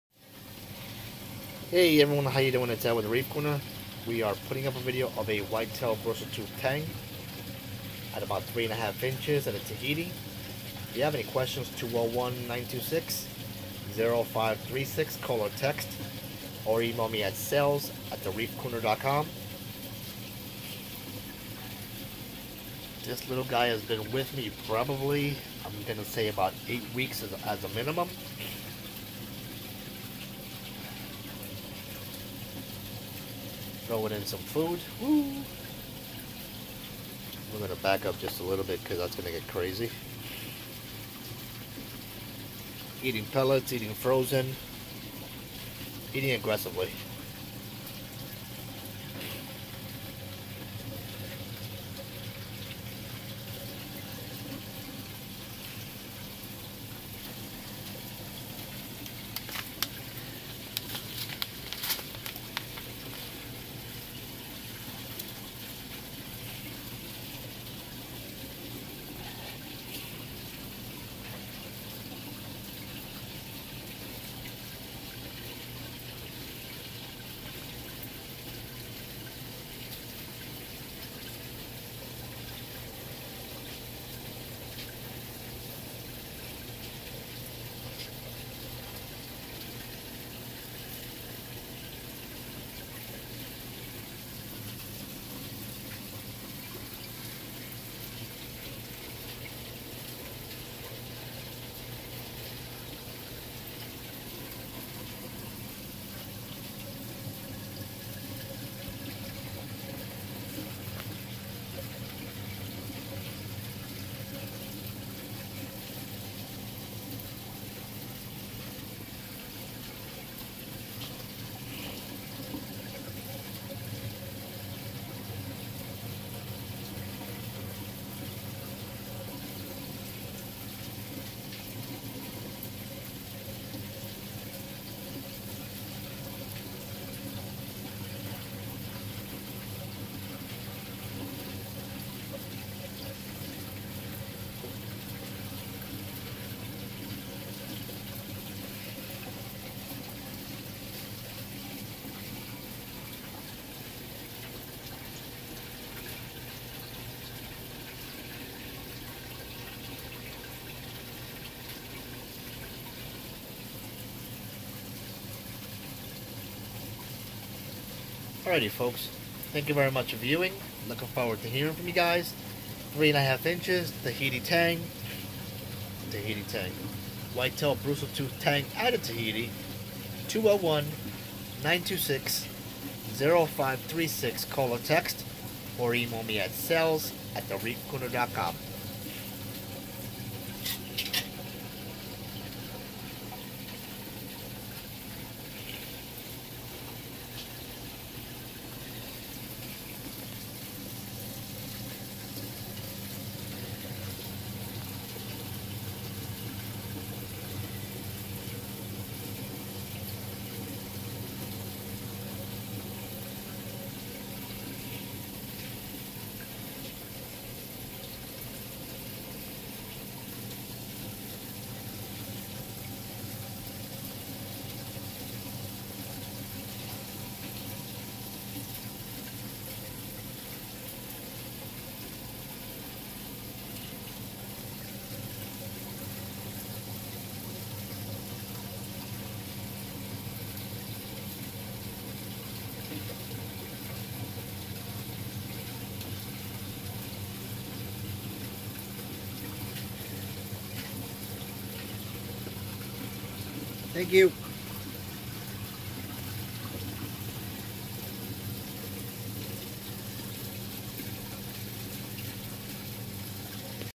Whitetail Bristletooth Tang $319 - Tahiti
4 inches and loves to eat.

Ed
the Reef Corner